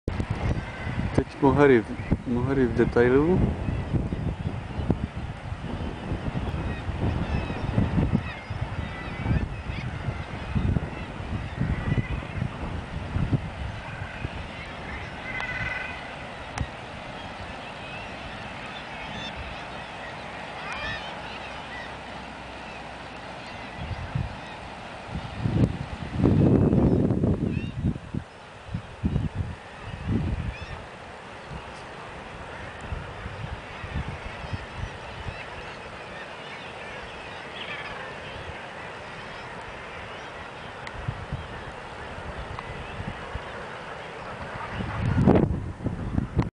Cliffs of Mohers in the morning
Cliffs of Mohers, Co. Galway, Ireland, in the morning. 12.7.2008